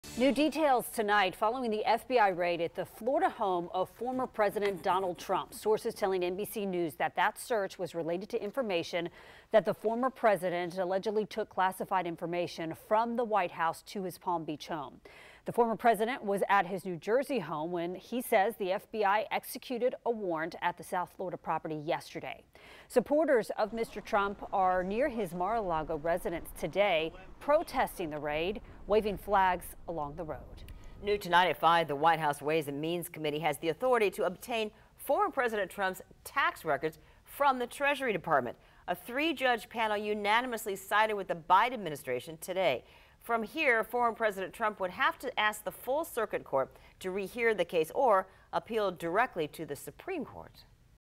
New details on the FBI raid at the Florida home of former President Donald Trump
Sources tell NBC News the search was related to information that the former President took from the White House.